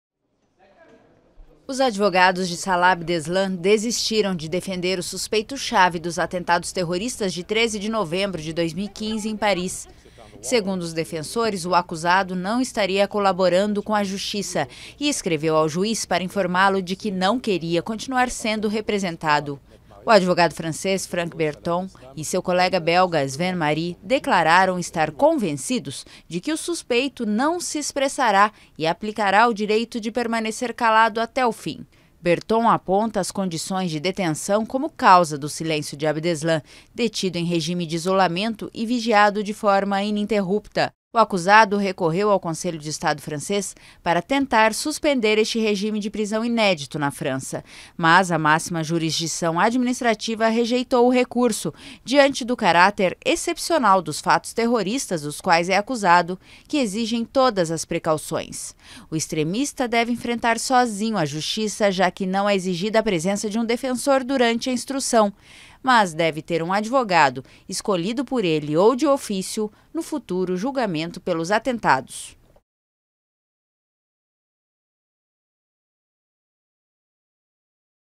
Advogados desistem de Abdeslam
Os advogados de Salah Abdeslam, suspeito-chave dos atentados terroristas de 13 de novembro de 2015 em Paris, desistiram de defender seu cliente, convencidos de que ele, preso em regime de isolamento, não colaborará com a justiça.